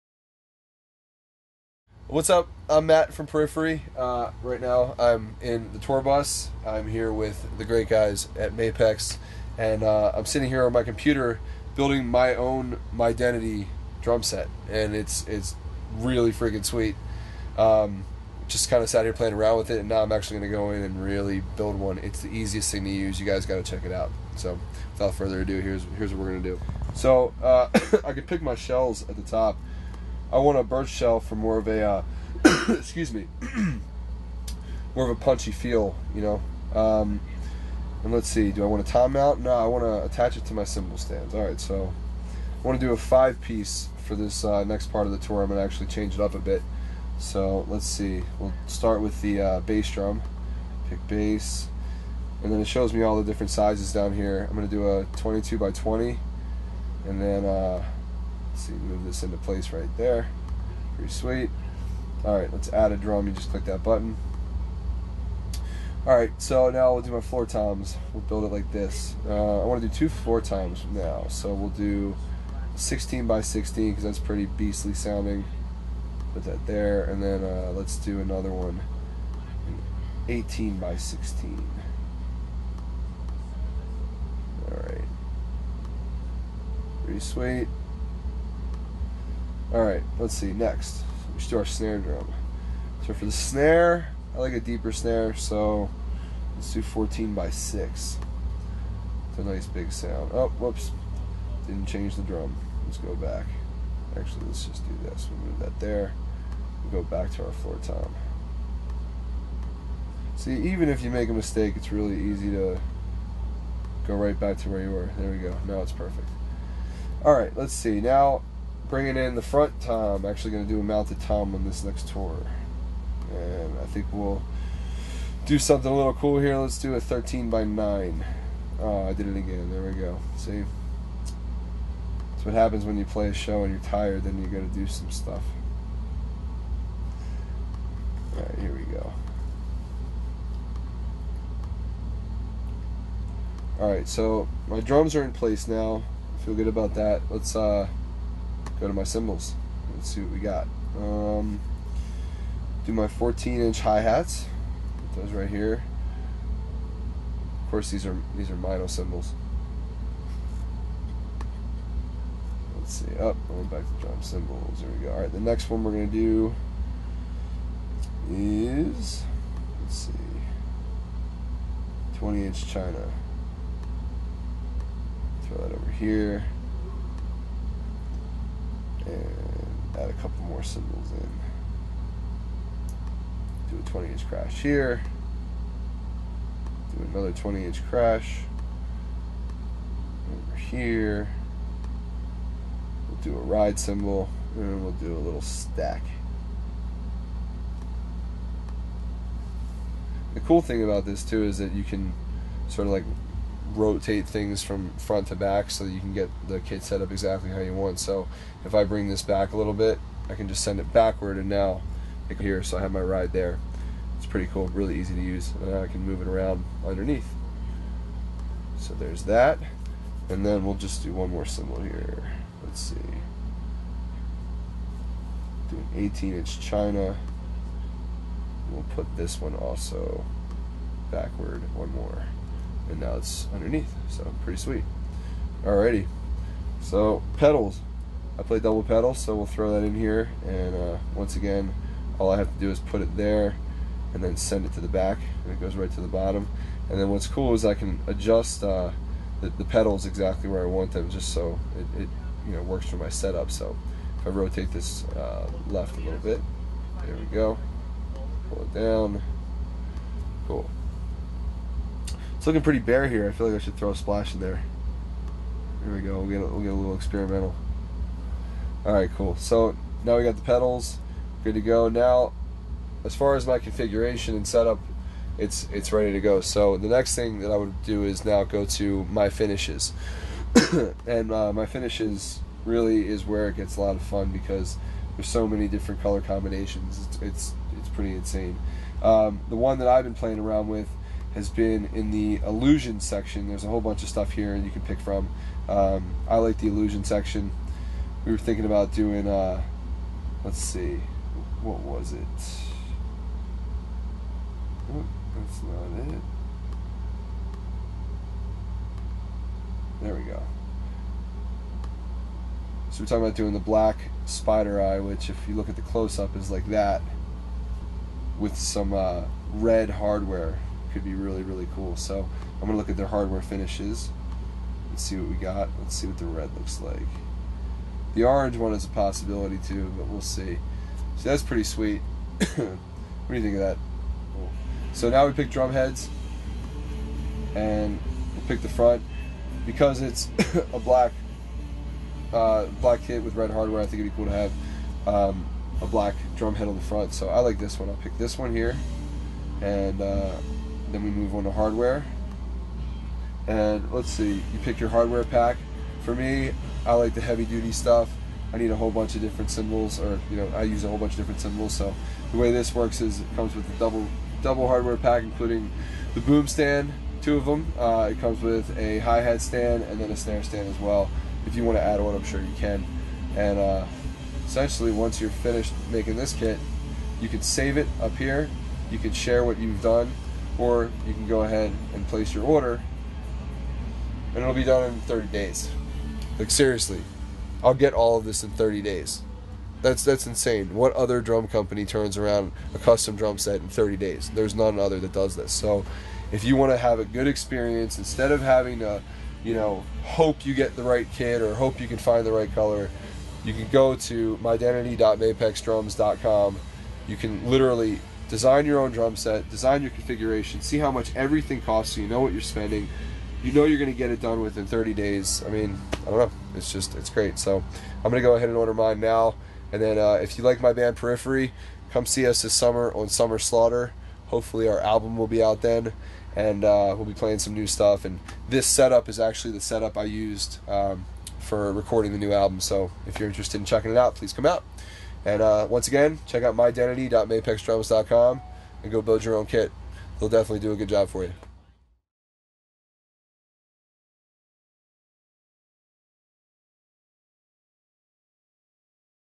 Matt Halpern - MyDentity Setup
Matt Halpern takes a break from his tour to set up his own kit on the Mapex MyDentity website! He shows how easy, quick, and beneficial it is to any drummer wanting a custom kit